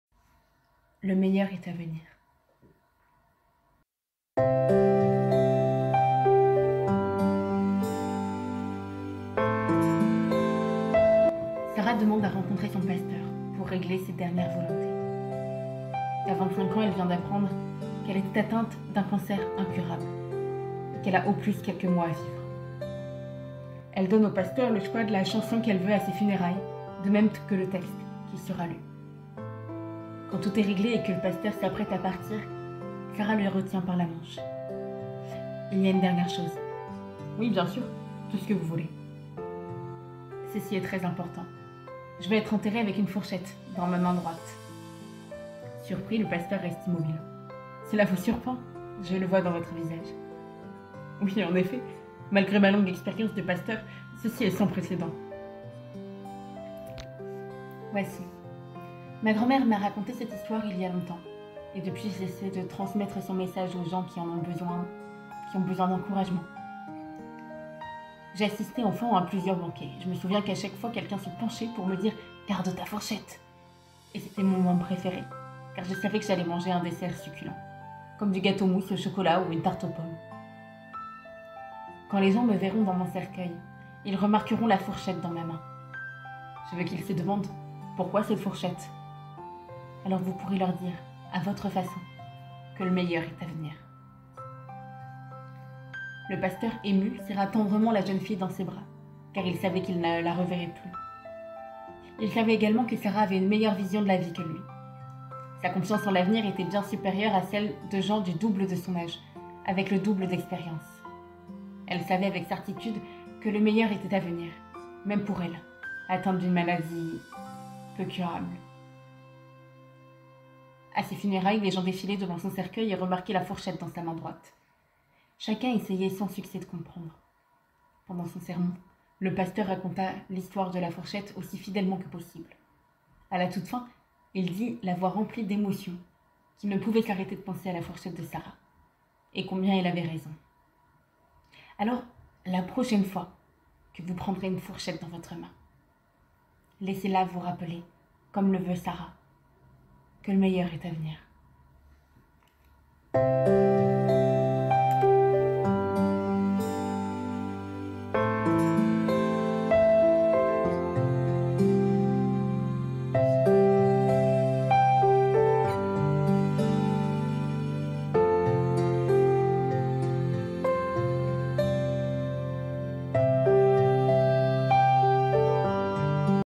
Le meilleur est à venir (Histoire courte)
Le 4 février, c'est la Journée mondiale de lutte contre le cancer, journée internationale consacrée à la prévention, la détection la lutte contre la stigmatisation et le traitement du cancer. Aujourd'hui, nous pensons à toutes ces personnes atteintes, en processus ou en espoir de guérison mais aussi à ceux qui ont perdu un proche de cette maladie.

Tombée récemment sur le site evolution-101, j'ai décidé de vous partager 50 histoires courtes et inspirantes qu'il rassemble.
Tous les lundis pour bien commencer la semaine ou les dimanches pour bien la terminer, passez quelques minutes à mes côtés pour reprendre espor en l'humanité.

Belle écoute et belle découverte

00:00 Introduction
00:15 Début de l'histoire
2:50Fin de l'histoire